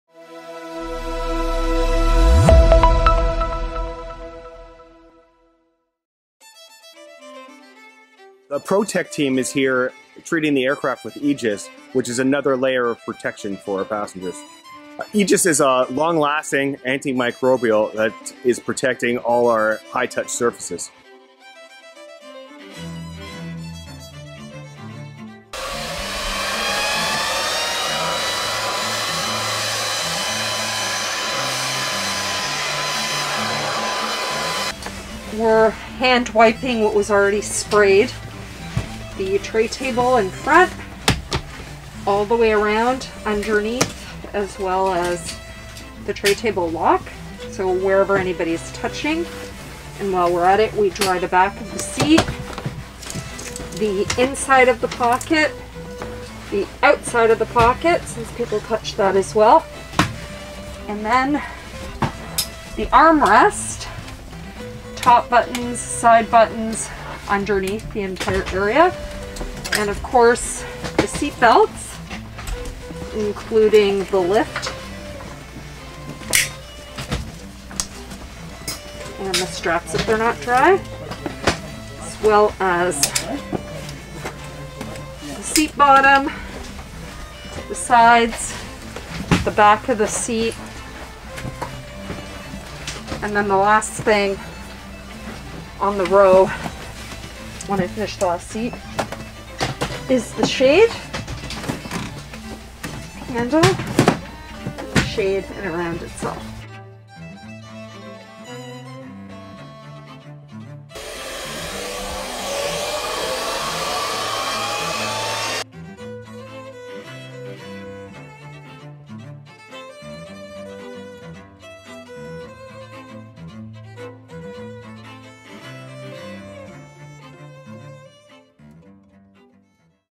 SUNWING - Surface Protection Program with AEGIS® Microbe Shield
SUNWING has taken steps to increase hygiene on its aircraft by using the AEGIS® Microbe Shield providing passengers with an extra level of protection when flying. The antimicrobial spray controls and prevents the growth of bacteria on surfaces.